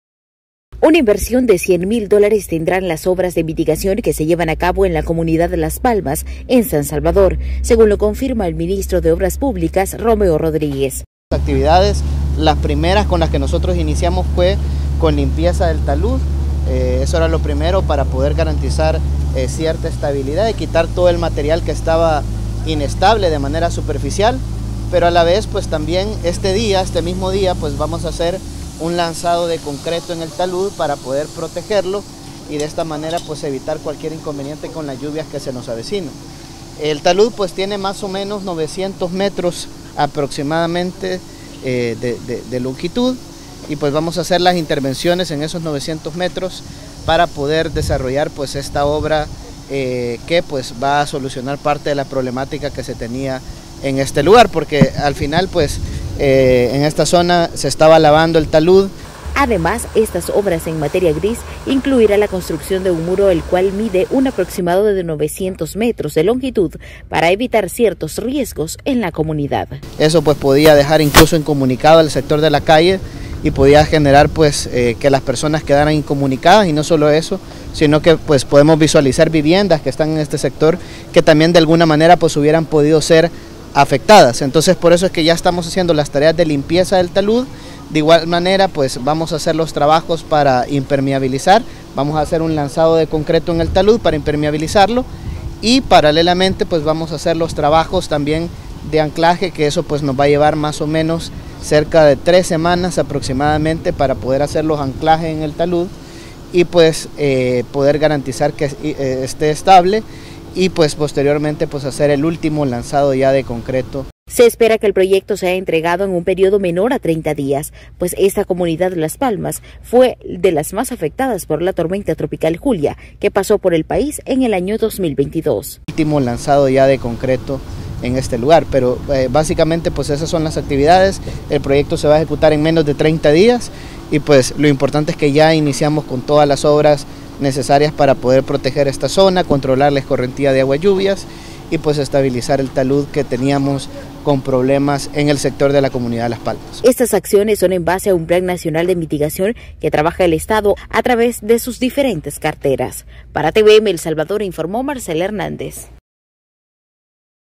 MOP ejecuta obra de mitigación en comunidad Las Palmas.
Más de 10,000 habitantes de la comunidad Las Palmas serán beneficiadas con una obra de mitigación que ejecuta el Ministerio de Obras Públicas.